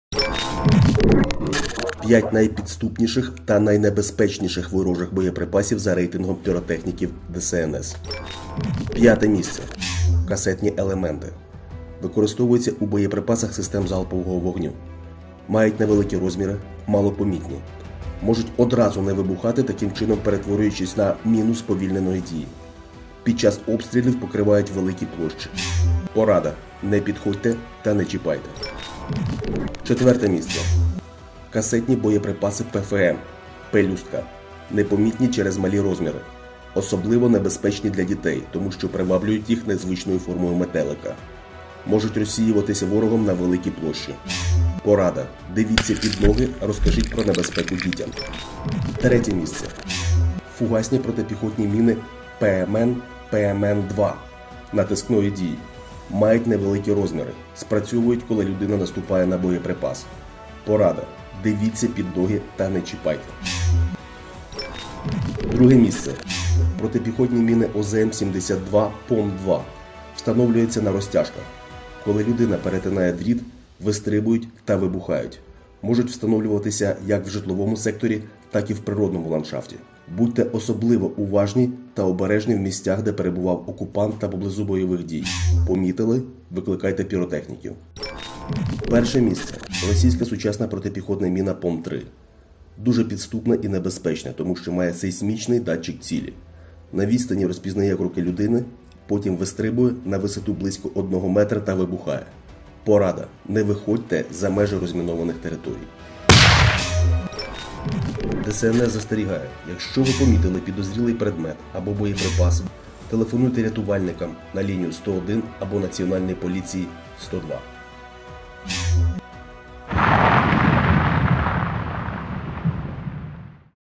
П’ять найпідступніших ворожих боєприпасів за рейтингом піротехніків ДСНС
5️⃣ Касетні елементи. Використовуються у боєприпасах систем залпового вогню. Порада: не підходьте та не чіпайте!

4️⃣ Касетні боєприпаси ПФМ (пелюстка).  Непомітні, через малі розміри. Особливо небезпечні для дітей, приваблюють їх незвичною формою метелика.  Порада: дивіться під ноги, розкажіть про небезпеку дітям!

3️⃣ Фугасні протипіхотні міни ПМН, ПМН2 натискної дії. Мають невеликі розміри. Порада: дивіться під ноги та не чіпайте!

2️⃣ Протипіхотні міни ОЗМ 72, ПОМ 2. Встановлюються на розтяжках. Коли людина перетинає дріт, вистрибують та вибухають. Будьте особливо уважні в місцях, де перебував окупант та поблизу бойових дій. Помітили - викликайте піротехніків!

1️⃣ Сучасна протипіхотна міна ПОМ 3. Має сейсмічний датчик цілі. На відстані розпізнає кроки людини, потім «вистрибує» на висоту близько одного метра та вибухає. Порада: не виходьте за межі розмінованих територій!

Помітили підозрілий предмет або боєприпаси, телефонуйте 101